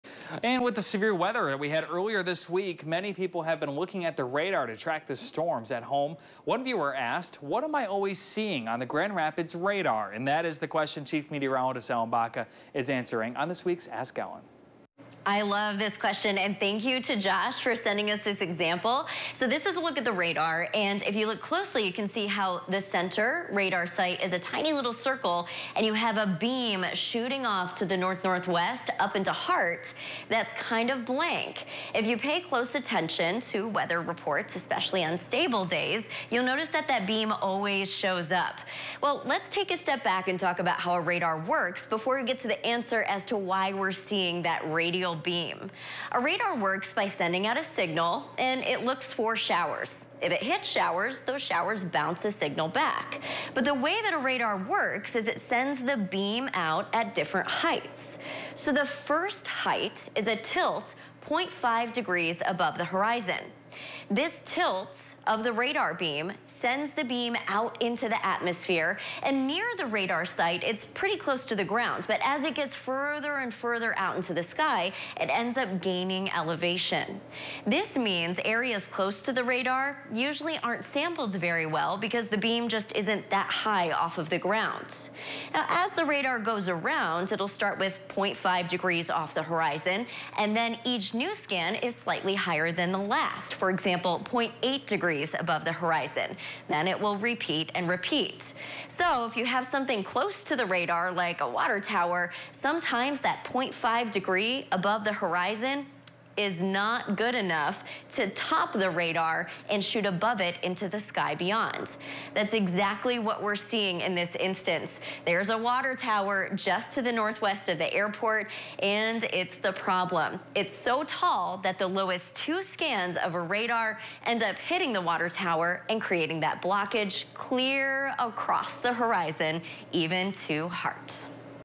Ask Ellen: What am I seeing on Grand Rapids radar?
There is a small blank spot on the Grand Rapids radar. Why? (July 16, 2024)